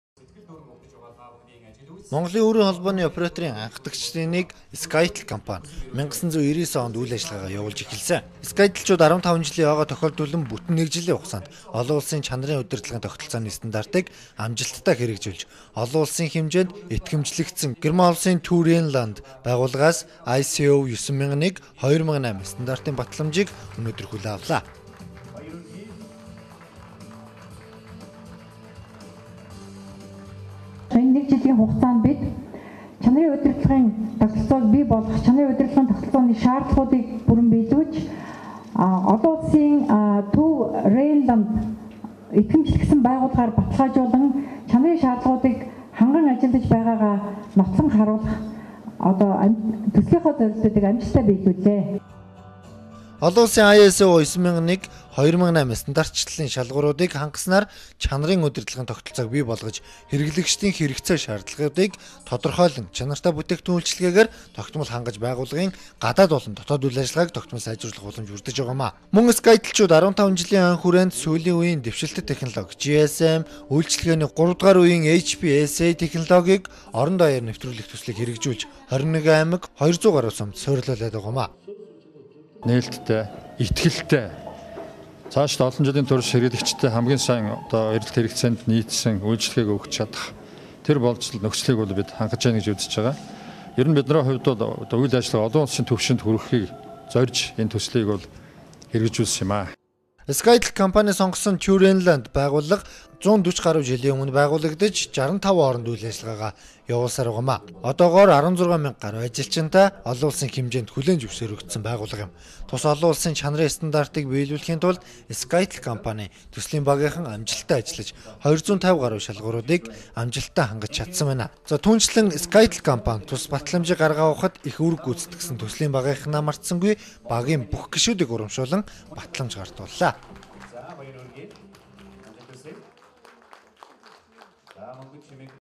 Skytel-Chanariin udirdlagiin togtoltsoonii ISO 9001:2008 standartiin batlamjaa avlaa
СКАЙТЕЛ КОМПАНИ ЧАНАРЫН УДИРДЛАГЫН ТОГТОЛЦООНЫ  ISO 9001:2008СТАНДАРТЫН ШААРДЛАГУУДЫГ ХАНГАЖ ОЛОН УЛСЫН БАТЛАМЖАА
ГАРДАН АВЛАА